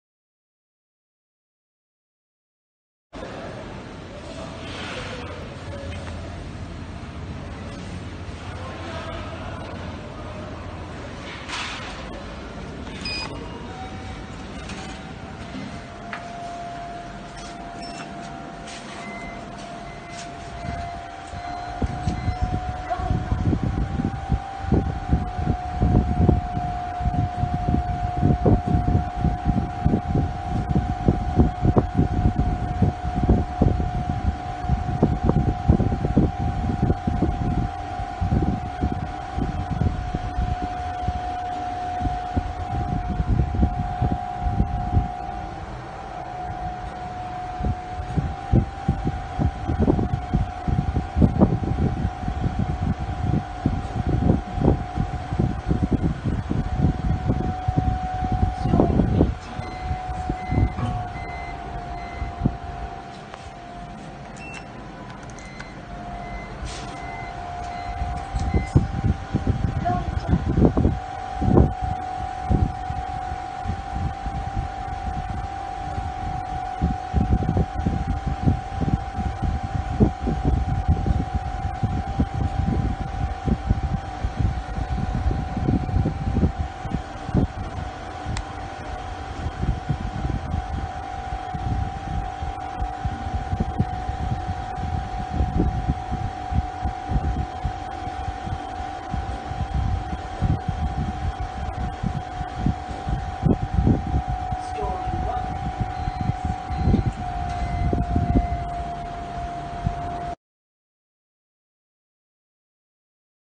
Block 262 Waterloo Street IFE Lift (Lift D)
Shaikabdulrahaman Shaikamanoli aka SG6229G film and record this lift as it is very new that started operation recently.

This video was recorded on 1st July 2022 Friday and this video was recorded by Shaikabdulrahaman Shaikamanoli aka SG6229G.